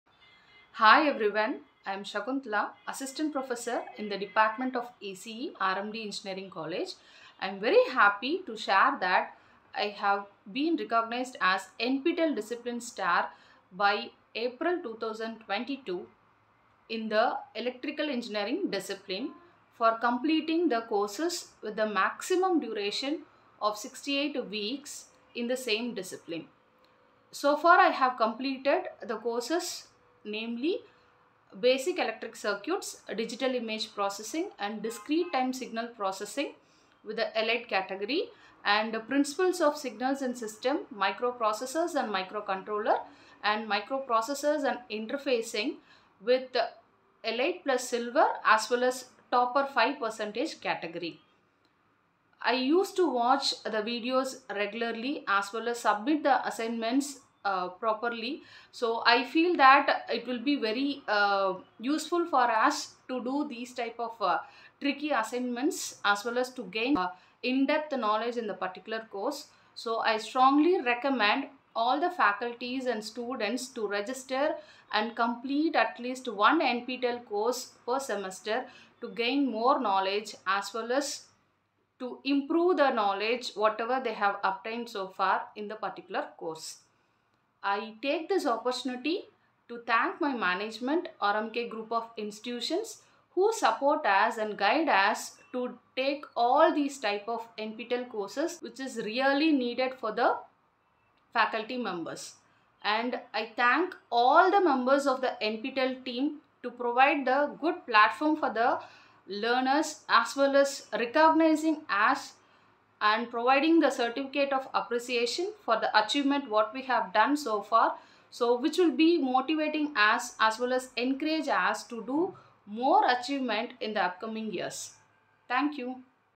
NPTEL DISCIPLINE STAR | Electrical Engineering NPTELStars @nptelindia
NPTEL DISCIPLINE STAR IN ELECTRICAL ENGINEERING DISCIPLINE.
COMPLETED THE COURSES NAMELY
* DISCRETE TIME SIGNAL PROCESSING:ELITE
* DIGITAL IMAGE PROCESSING:ELITE
* BASIC ELECTRIC CIRCUITS :ELITE
* PRINCIPLES OF SIGNALS AND SYSTEMS (TOPPER 5%): Elite+Silver
* MICROPROCESSORS AND MICROCONTROLLERS(TOPPER 5%): Elite+Silver
*MICROPROCESSORS AND INTERFACING (TOPPER 5%):Elite+Silver